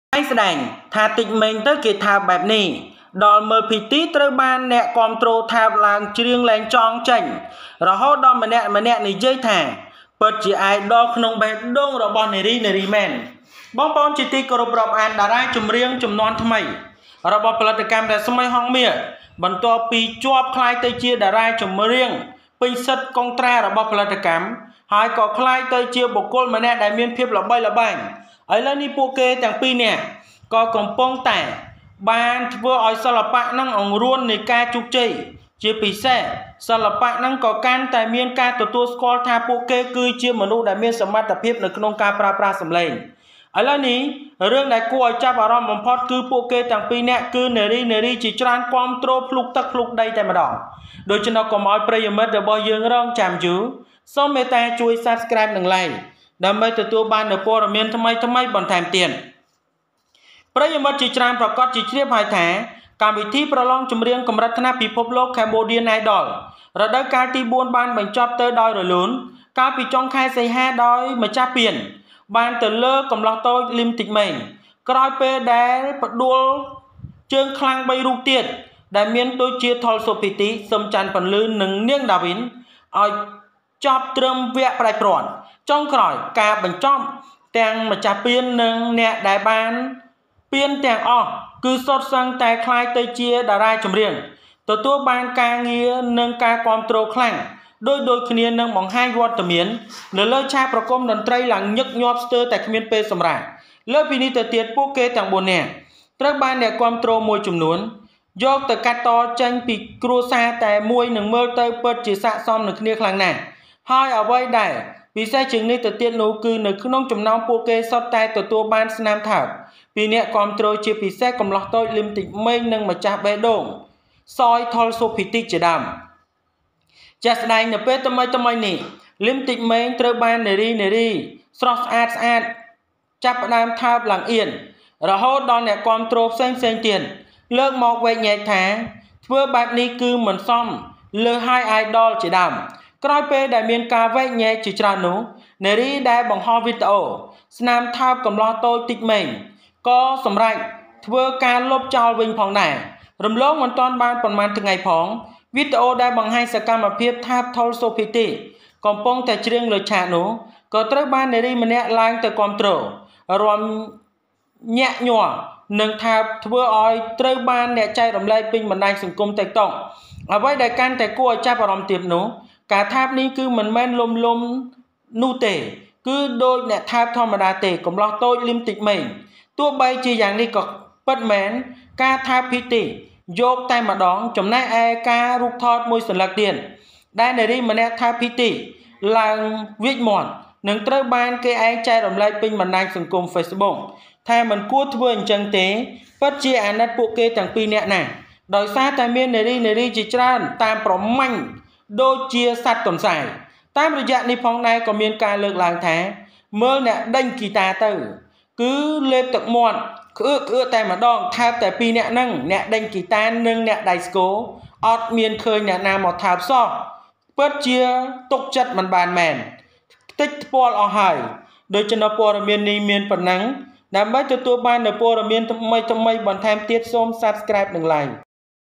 ស្ដែងៗតិចម៉េង ទៅ​គេ​ថើប​បែបនេះ, ដល់​មើល​ភិទិ ត្រូវ​អ្នក​គាំទ្រ​ថើប​ឡើង​ច្រៀង​លែង​ចង់​ចេញ​រហូតម្នាក់ៗ
ស្ដែងៗ តិចម៉េង ទៅ​គេ​ថើប​បែបនេះ, ដល់​មើល​ភិទិ ត្រូវ​អ្នក​គាំទ្រ​ថើប​ឡើង​ច្រៀង​លែង​ចង់​ចេញ​ រហូតម្នាក់ៗ Apparently, less than a few people kissed like this, to see the fans kissed and sang, no longer want to go out, everyone.